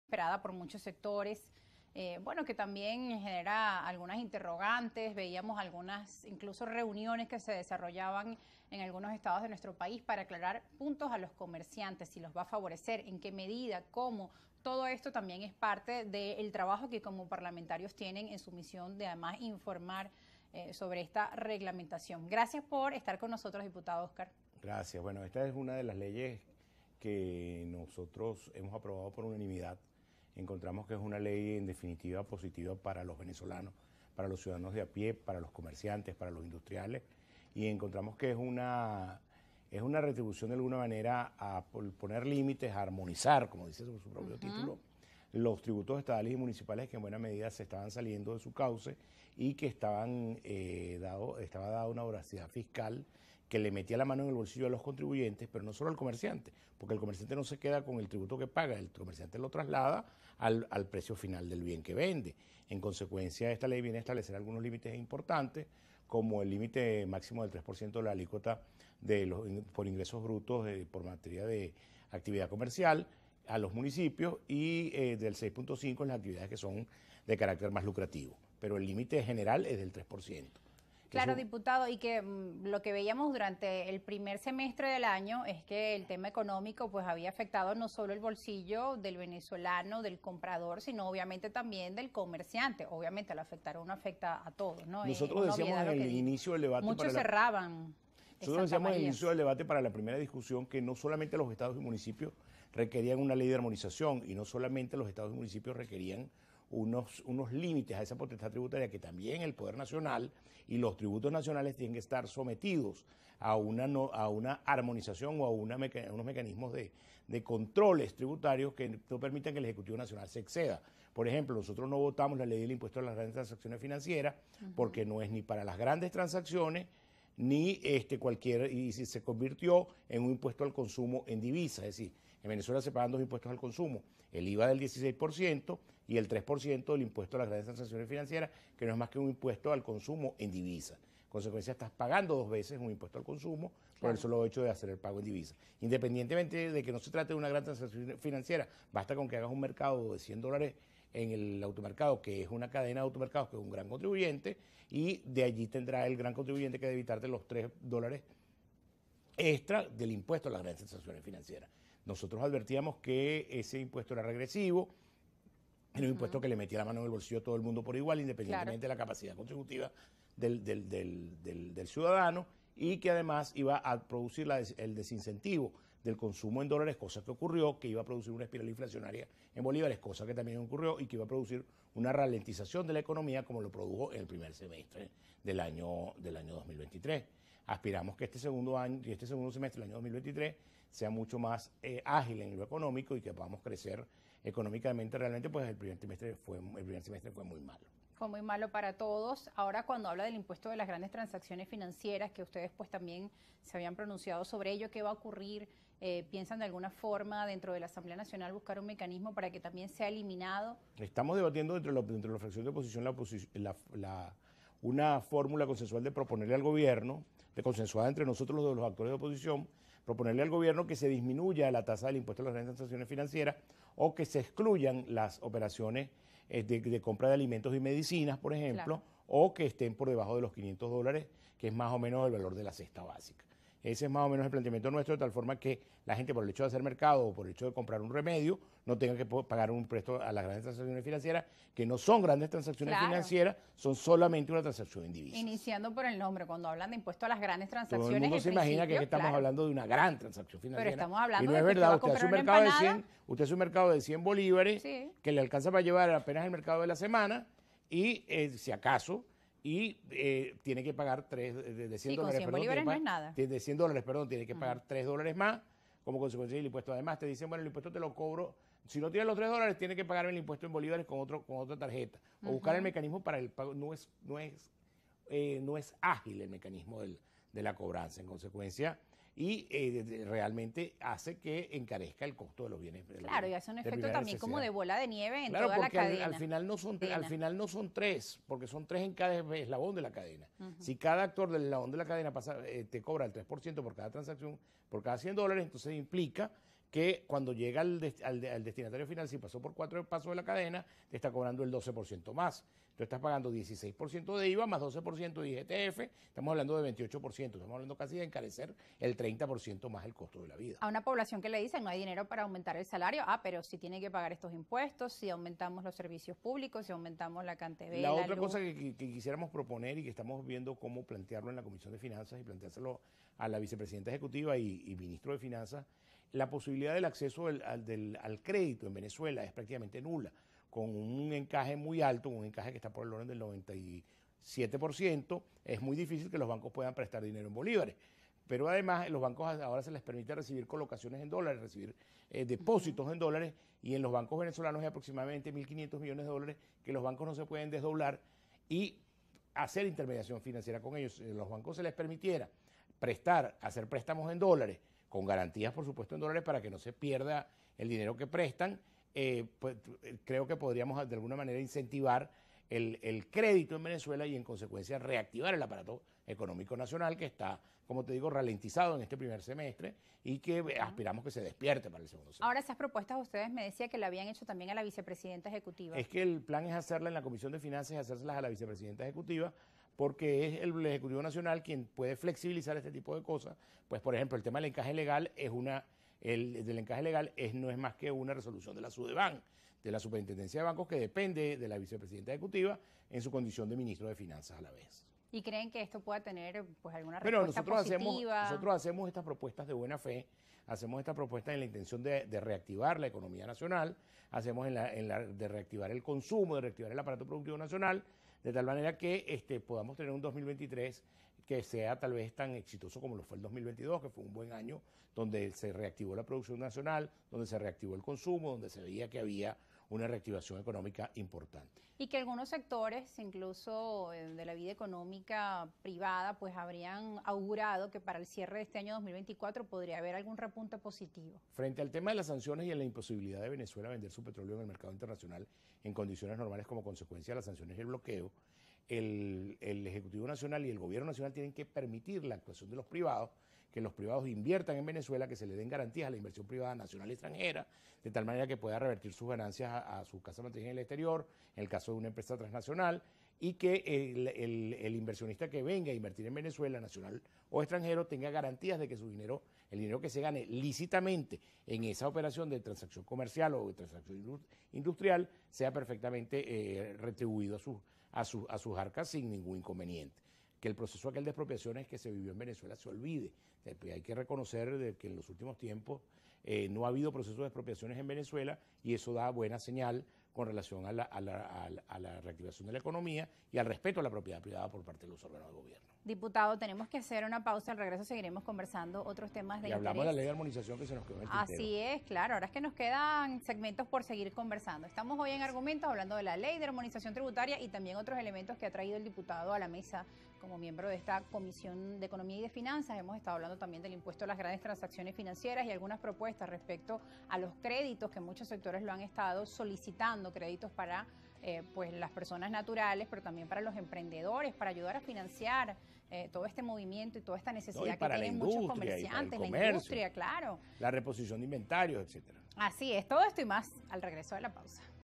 GV Argumentos | Entrevista con Óscar Ronderos, Diputado de la Asamblea Nacional (2/5)
GV Argumentos